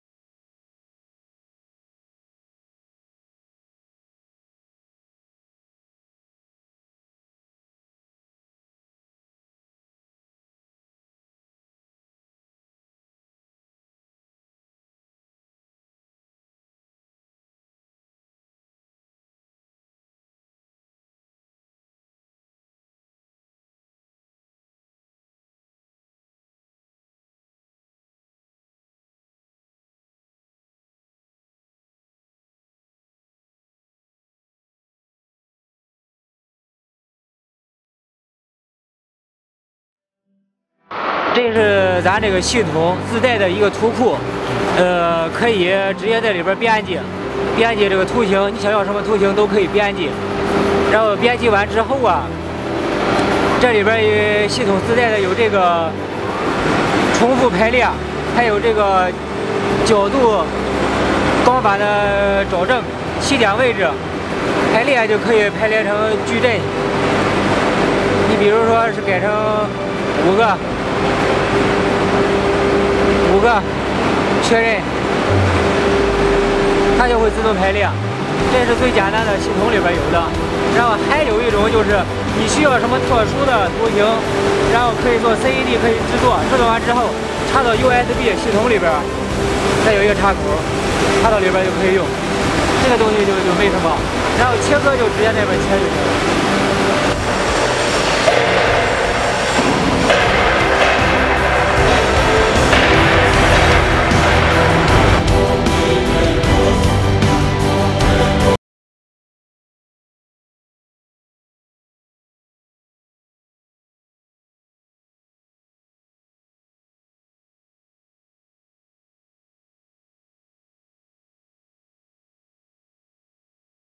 KR-PL CNC Gantry Profiling Cutting Machine
It is a kind of high-efficiency steel plate automatic cutting  equipment developed by our company for the cutting of various shapes of metal sheets. It adopts gantry structure type, bilateral drive with gear rack, It has the characteristics of stable structure, good rigidity and high speed, hollow box design and good heat dissipation.